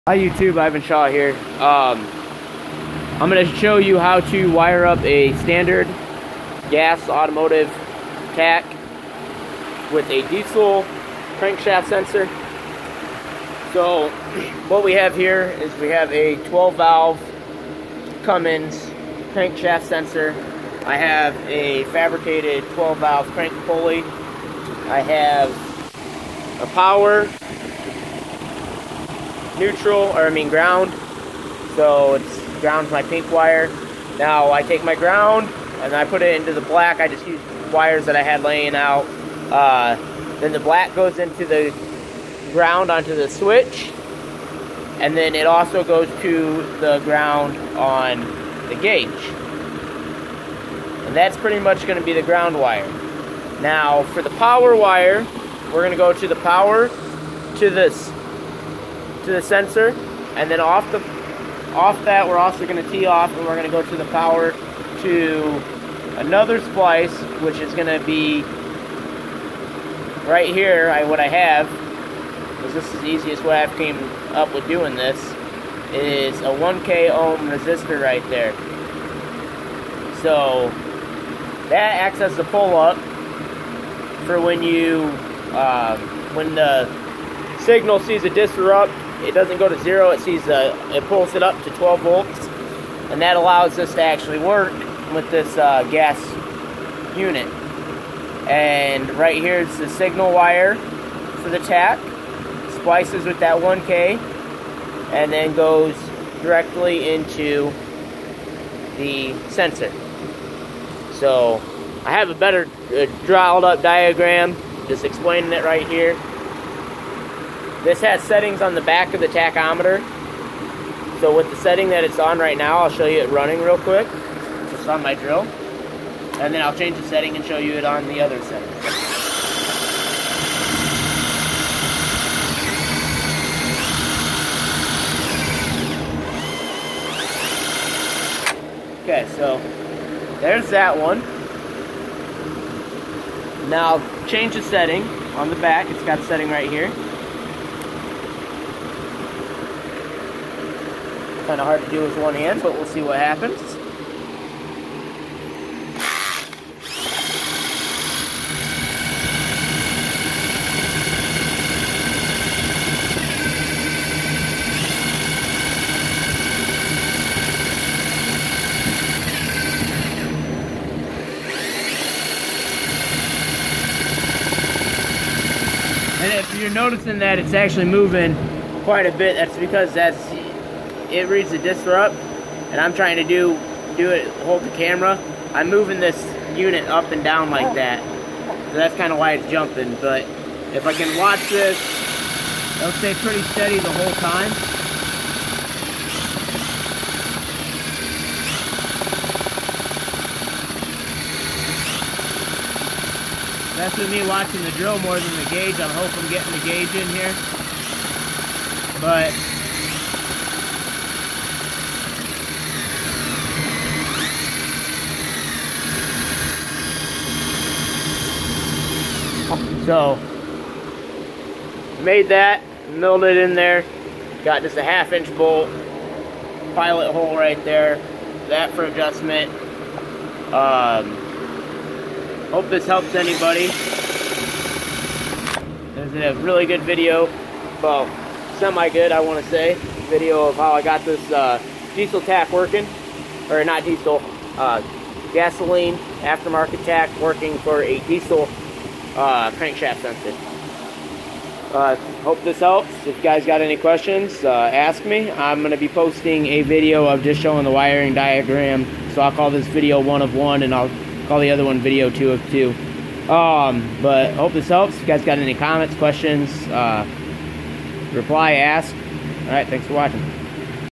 Gas tach diesel (tachometer) running off Dodge Cummins crank shaft sensor
This is how I ran mine in my 1973 international swap wanted to show others be careful  thow ic wired backwards can fri the sensor  you will need to know what wires are ground  hot and signal also if you sensor can handle 12 volts   hope this helps any diesel or electronic question just ask